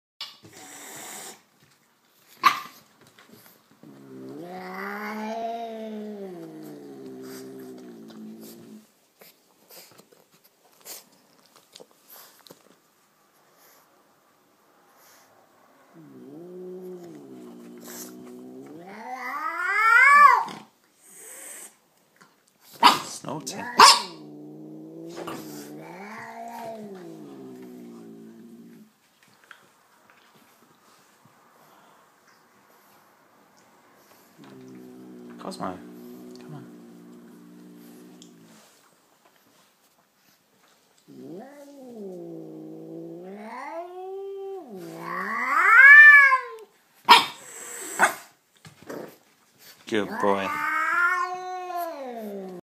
Gato Persa enfadado - Angry Persian cat
Este gato Persa color chocolate, está bastante enfadado. ¿Cuál será la razón de su enfado?

Este es el canal de la web de referencia sobre gatos

This chocolate-colored Persian cat is quite angry. What will be the reason for your anger?